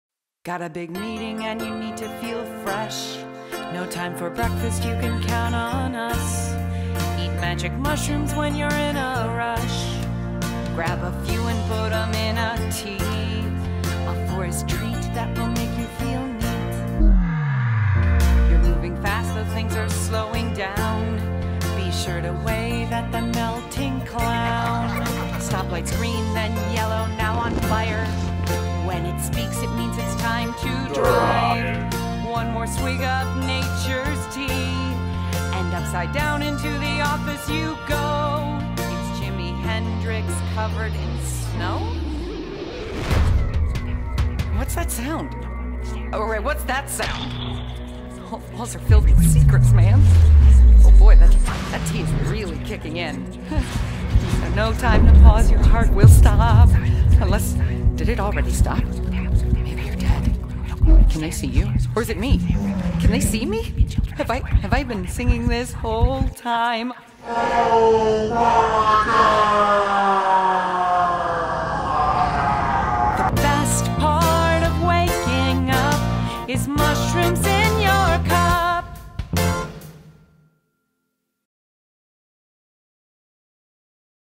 Mushroom Tea!
Need a little morning pick me up? Grab a cup of mushroom tea and let the magic of the forest take you away!

Directed by Jeff Tomsic
Produced by Rosie Kaller

Starring Nic Wegener
VO by Heather Anne Campbell
Featuring Jeff Sloniker as Melting Clown and Zora Bikangaga as Jimi Hendrix
Music Score by Mike Costantini

DP - Kevin Stewart
Steadicam - David Bladwin
Gaffer - Jordan Downey
Set Dresser - Ellie McElvain

or Amazon